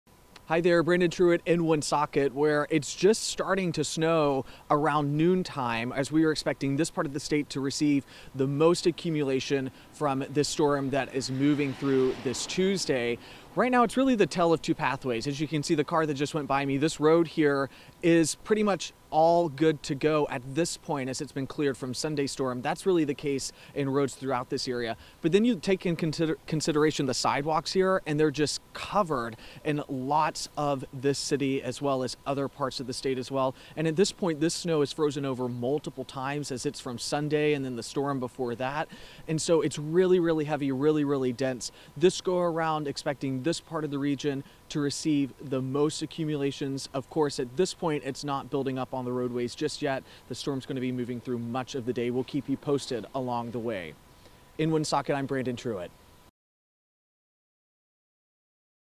VIDEO NOW: Brandon Truitt with a look at the conditions in Woonsocket - 12 p.m.
Brandon Truitt with a look at the conditions in Woonsocket - 12 p.m.